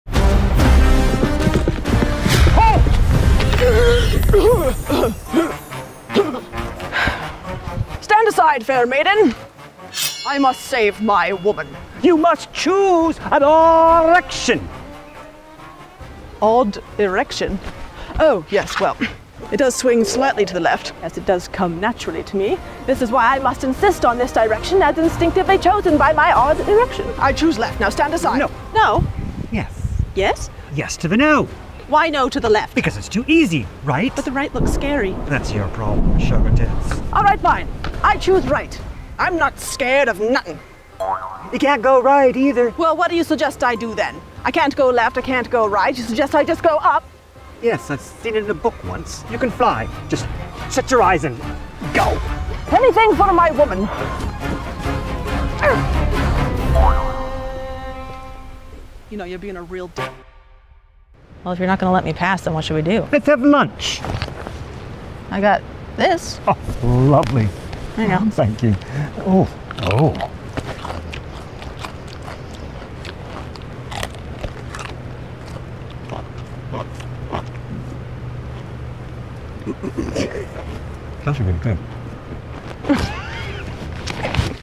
*BANNED* 1 Minute Comedy Skit Short Film TO THE LEFT "UNCUT" version
Vicky Smudge as The Gaytekeeper
Kaliandra Capri as Sir Gartits

We made this for a 1-minute mobile film challenge with the keywords "Choose a Direction". We chose an odd'ERECTION.